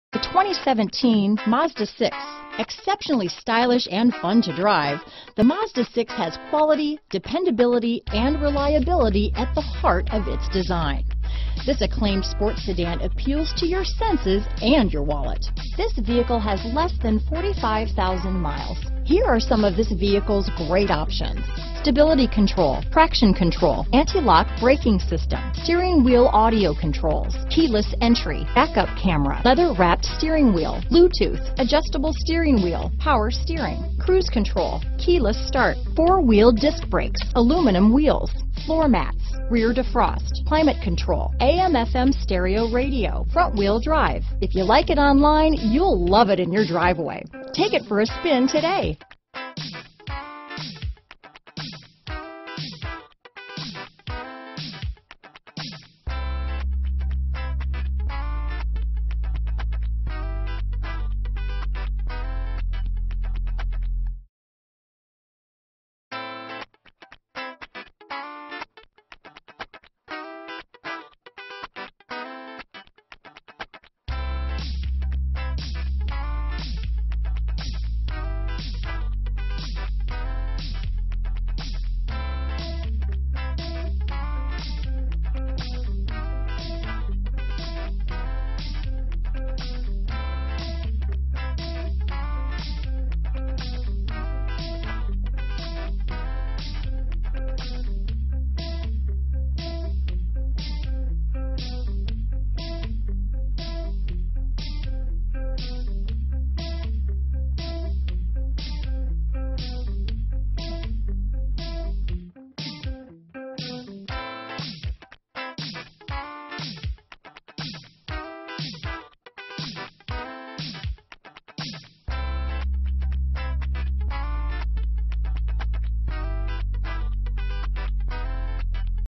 2017 Mazda Mazda6 Reno, Carson City, Northern Nevada, Sacramento, Elko, NV 54172
Soul Red Metallic Used 2017 Mazda Mazda6 availalble in Reno, Nevada at Internet Auto Reno. Servicing the Carson City, Northern Nevada, Sacramento, Elko, NV area.

Look at this 2017 Mazda Mazda6 Touring. Its Automatic transmission and Regular Unleaded I-4 2.5 L/152 engine will keep you going. This Mazda Mazda6 features the following options: SOUL RED METALLIC PAINT CHARGE, SOUL RED METALLIC, BLACK, LEATHERETTE SEAT TRIM, Window Grid Antenna, Wheels: 19 Alloy, Valet Function, Trunk Rear Cargo Access, Trip computer, Transmission: 6-Speed Automatic SKYACTIV-DRIVE -inc: straight shifter, manual shift mode and sport mode button, and Tires: P225/45R19 AS. Test drive this vehicle at Internet Auto Rent & Sales, 1220 Kietzke Lane, Reno, NV 89502.